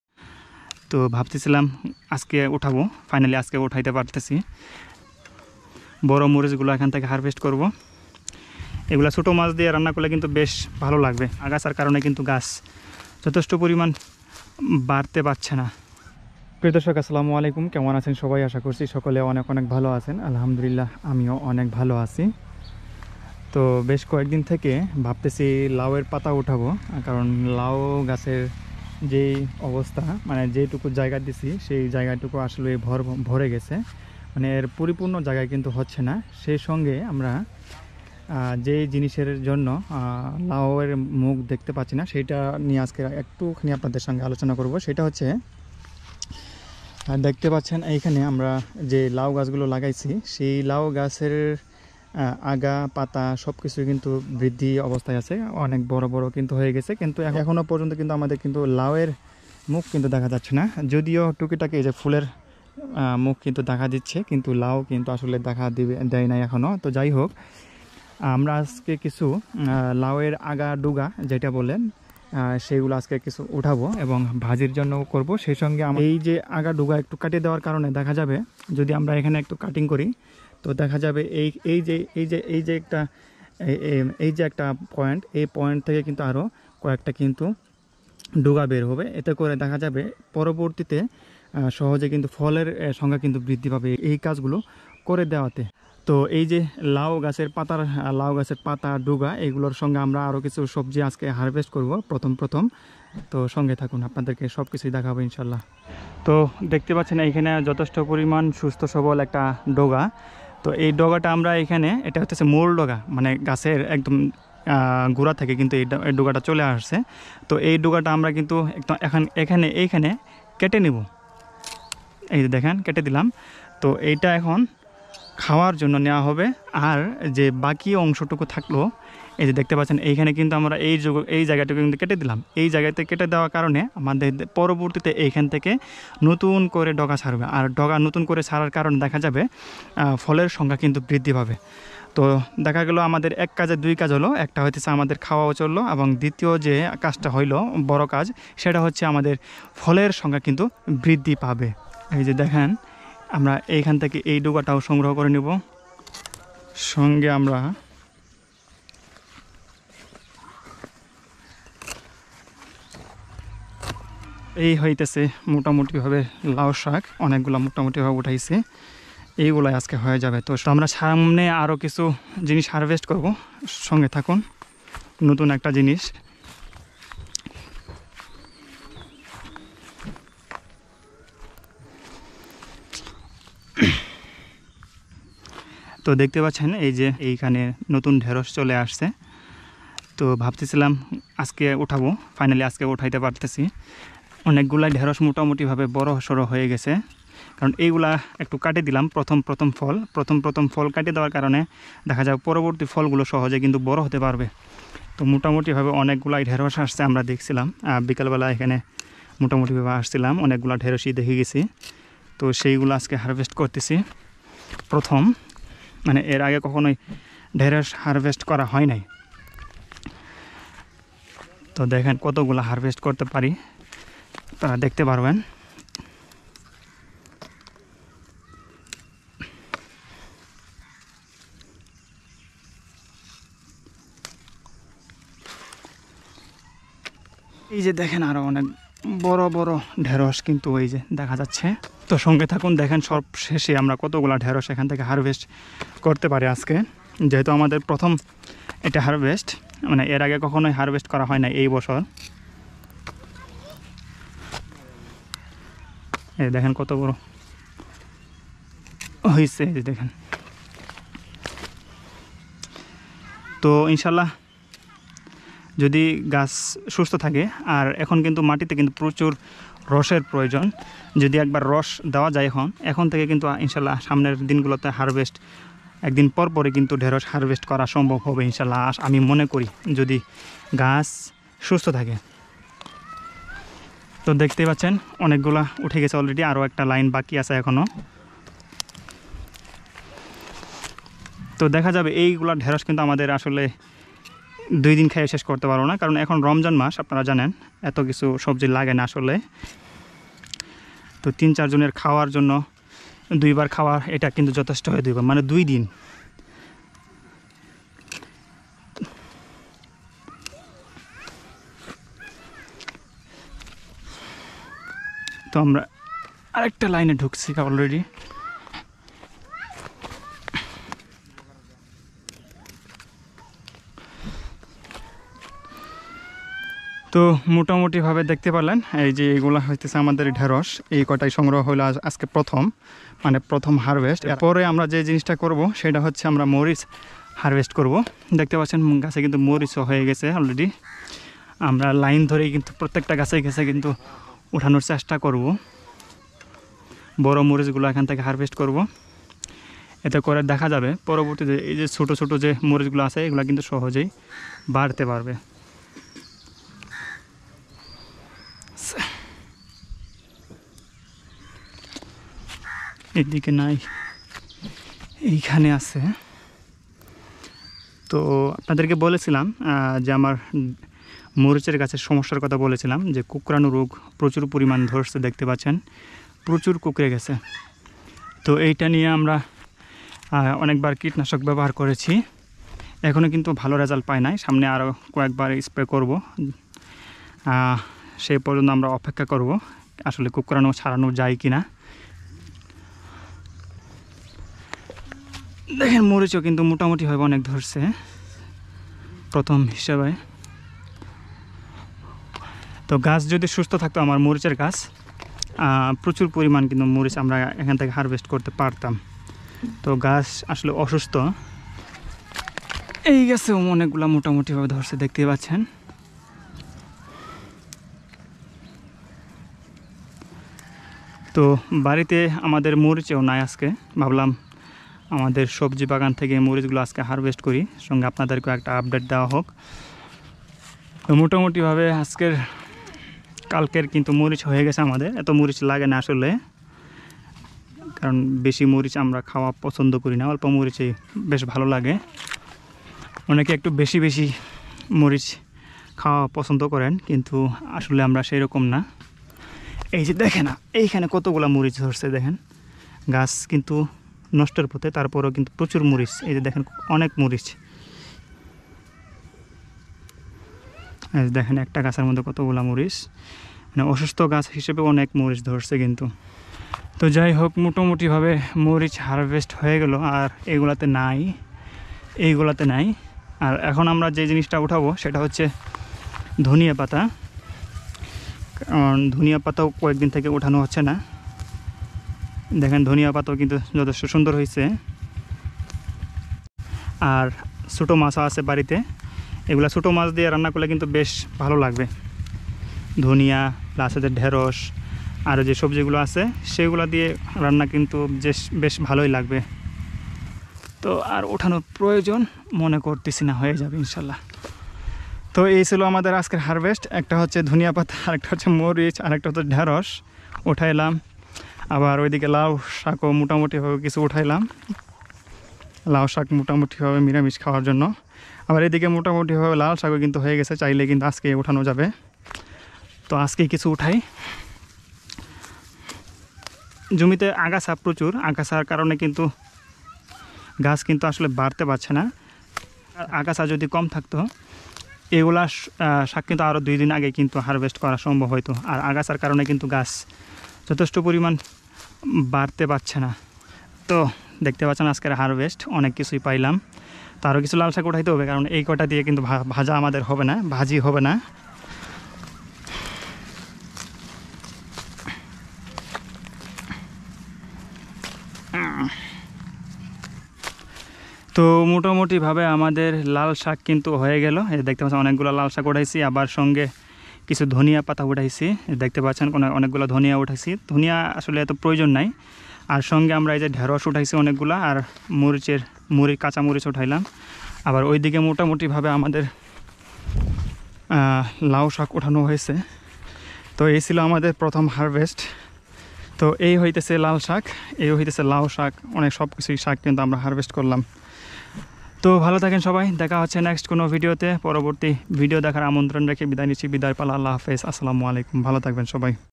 আজকে বাগানের প্রথম সংগ্রহ | Today is the first vegetable harvested from the garden
আজকে আমার সবজি বাগানের প্রথম সংগ্রহ করা ঢেঁড়স মরিচ ধনিয়া পাতা লাউ শাক লাল শাক
Today is the first vegetable harvested from the garden